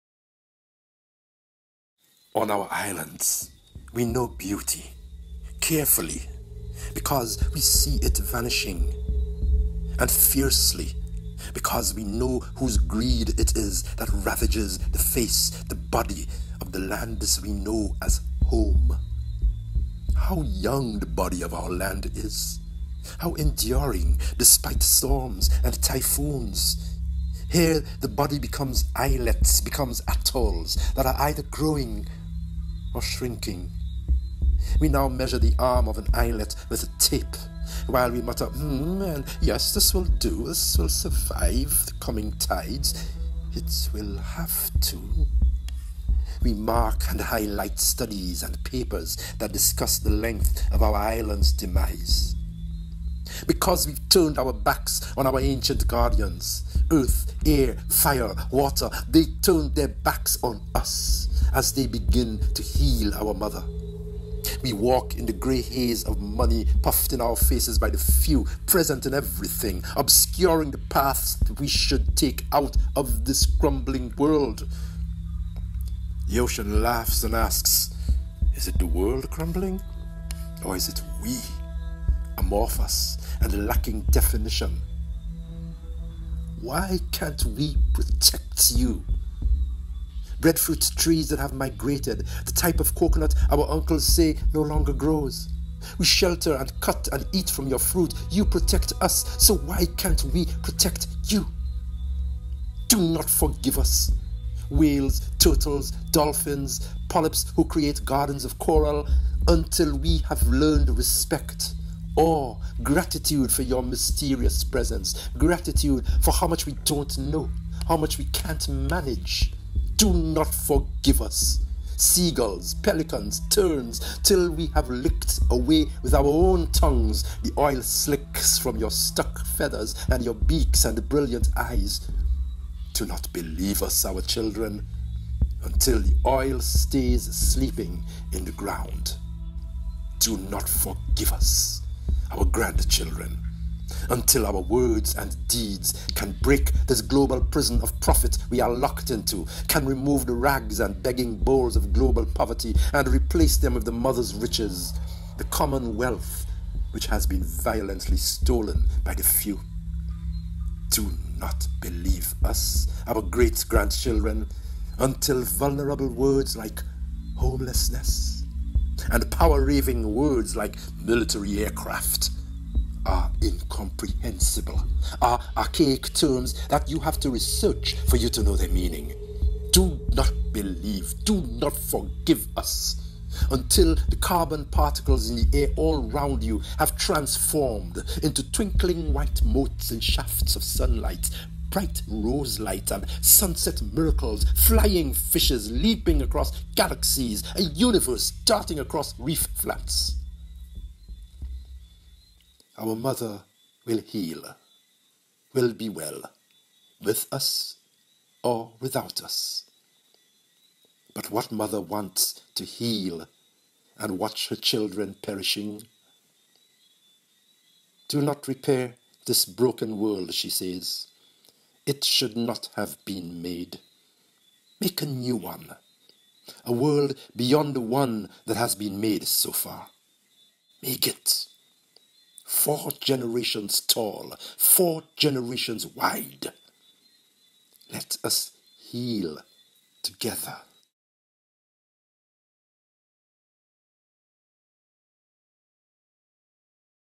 On Our Islands: Kendel Hippolyte & Kathy Jetn̄il-Kijiner
A spoken word reflection by Kendel Hippolyte of Saint Lucia with contributions by Kathy Jetn̄il-Kijiner of the Marshall Islands on the occasion of the 30th Anniversary of the Alliance of Small Island States (AOSIS) - December 3, 2020.

Video Footage courtesy:
UNDP
Maldives Marketing and PR Corporation
Belize Tourism Board
Mot Mot Films
John Bakari Davidson